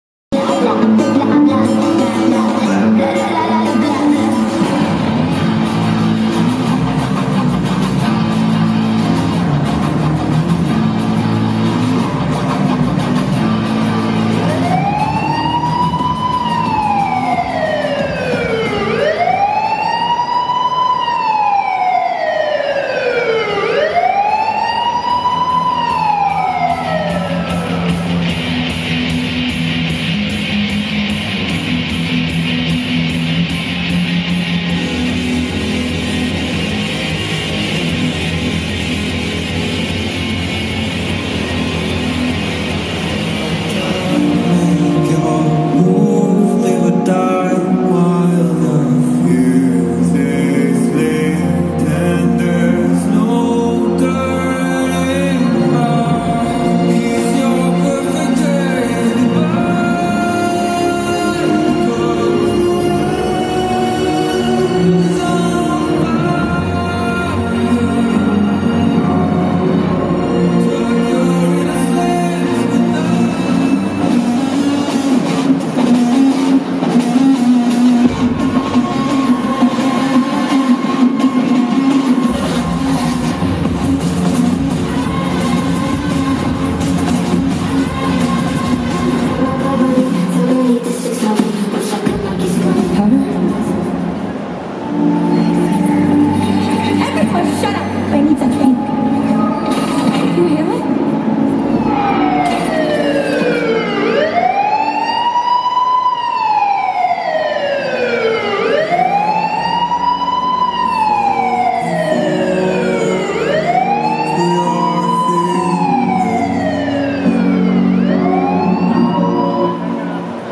Manga Barcelona 2022 - Concurso cosplay performance - Jinx y Vi (League of legends) segundo premio
Manga Barcelona 2022 - Concurso cosplay performance

Actuación: Jinx y Vi (League of legends)
Premio: segundo puesto